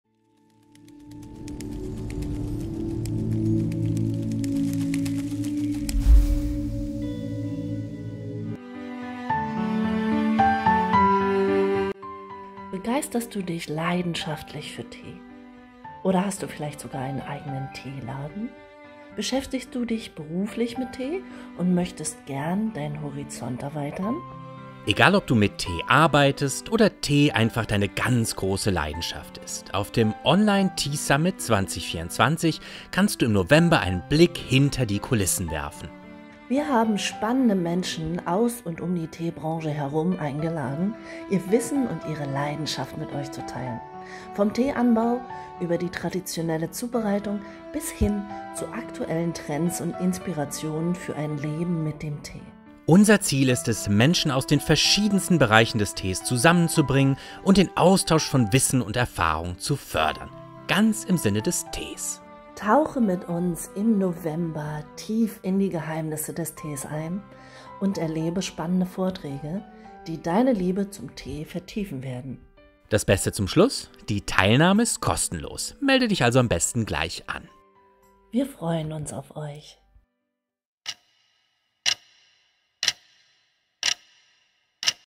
Tea Summit 2024 – Deine Reise in die Welt des Tees!
Vom 14. bis 17. November 2024 kannst Du beim Tea Summit 2024 tief in die Tee-Kultur eintauchen! Erlebe vier Tage voller wertvollem Wissen, geteilt von internationalen Teeexperten. Täglich werden 2 Videos veröffentlicht, die Dir helfen, Dein Teewissen zu vertiefen. Die kostenlose Anmeldung ist ab dem 1. Oktober möglich. Sei dabei und entdecke Deine Liebe zum Tee neu! 🌿🍵 Jetzt kostenlos registrieren!